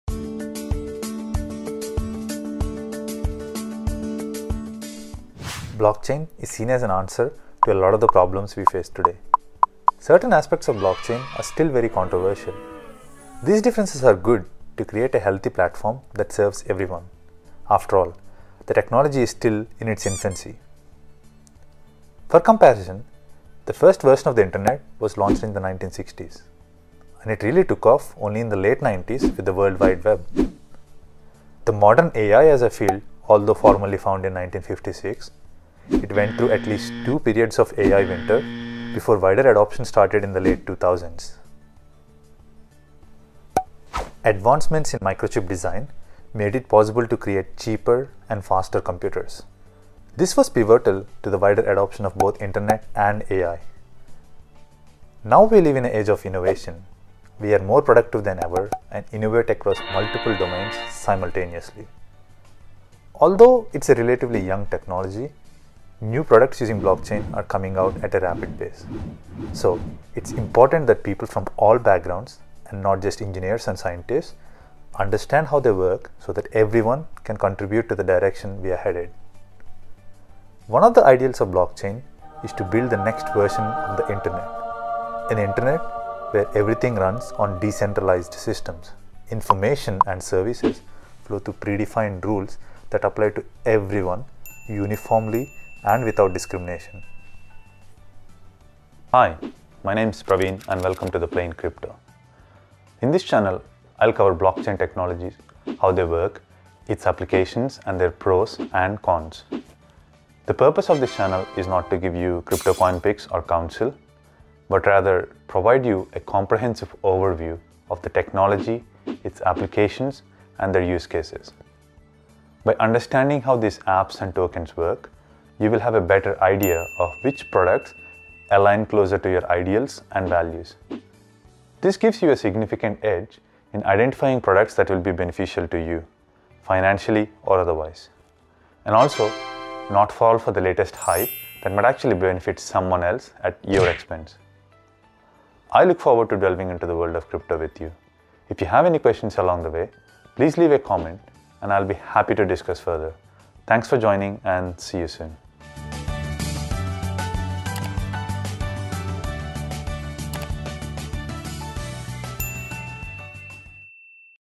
Welcome to the Plain Crypto
Chapters:
00:00 Introduction
00:23 History of Internet and AI
01:02 Importance of Blockchain
01:38 What's this channel about?

Blockchain is seen as an answer to a lot of the problems we face today!

Certain aspects of blockchain are still very controversial. These differences are good to create a  healthy platform that serves everyone. After all, the technology is still in its infancy.

For comparison:
- The first version of Internet was launched in 1960s and it really took off only in the late 90s with the World Wide Web.
- The modern AI as a field, although formally found around 1956, it went through at least 2 periods of “AI Winter” (1974–80 and 1987 to 93) before wider adoption started in the late 2000s.

Advancements in micro chip design made it possible to create cheaper and faster computers. This was pivotal to the wider adoption of both Internet and AI.

Now we live in age of innovation. We are more productive than ever and innovate across multiple domains simultaneously!

Although it’s a relatively young technology, new products using blockchain are coming out at a rapid pace. So, it’s important that people from all backgrounds, and not just engineers and scientists, understand how they work so that everyone can contribute to the direction we are headed.

One of the ideals of blockchain is to build the next version of the Internet. An internet where everything runs on decentralized systems. Information and services flow through pre-defined rules that apply to everyone uniformly, and without discrimination!

Hi, my name’s Praveen and welcome to The Plain Crypto. In this channel, I will cover blockchain technologies: how they work, its applications and their pros and cons.

The purpose of this channel is not to give you Crypto coin picks or council but rather to provide a comprehensive overview of the technology, applications and their use cases. By understanding how these apps and tokens work, you will have better idea of which products align closer to your ideals and values. This gives you a significant edge in identifying products that will be beneficial to you (financially or otherwise) and not fall for the latest hype that might actually benefit someone else; at your expense.

I look forward to delving into the world of crypto with you. Thanks for joining and see you soon!